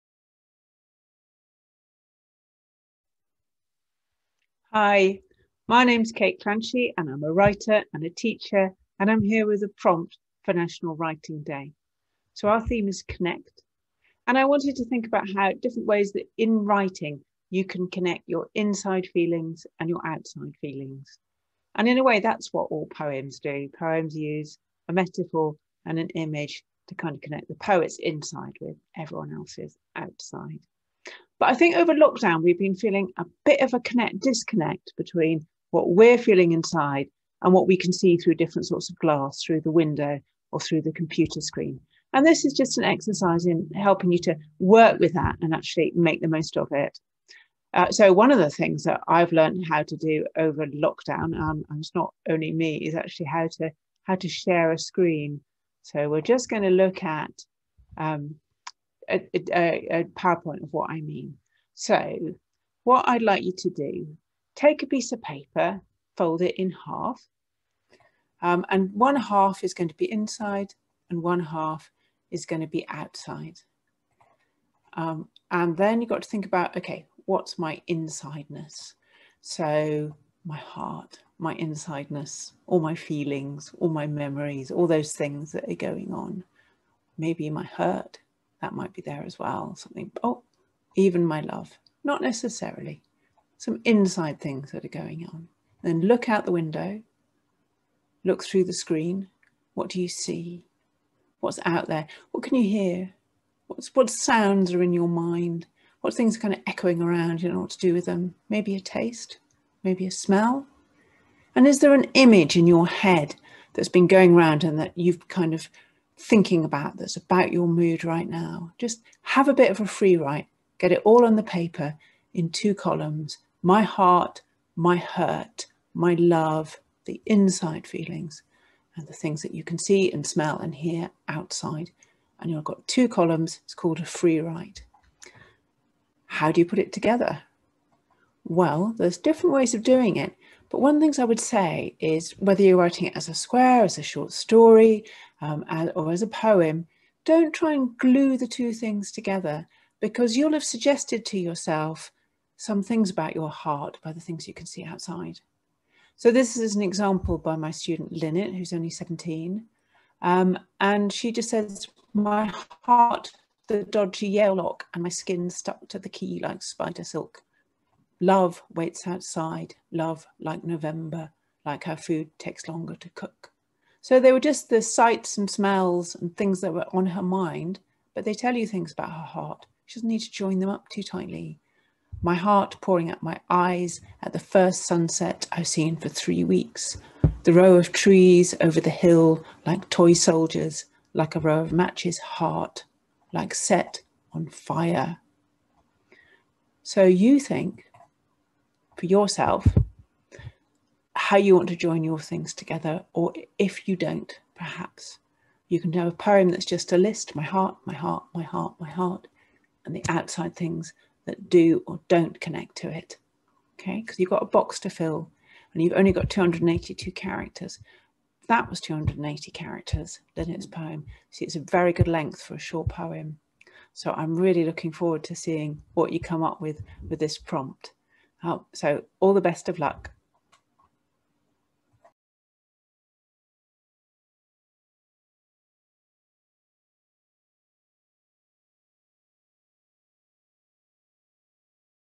Kate Clanchy on the FilltheBox challenge
Kate Clanchy, teacher and award-winning author of 'Some Kids I Taught and What They Taught Me' gives her advice on the FilltheBox challenge.

Complete the challenge by following these simple rules:

1. To the theme of connection, use Twitter's 280 character limit, a post-it note sized box or First Story's templates to write a story, letter or poem
2. Share your story online using FilltheBox, NationalWritingDay and @FirstStory
3. Tag one person to add their story to yours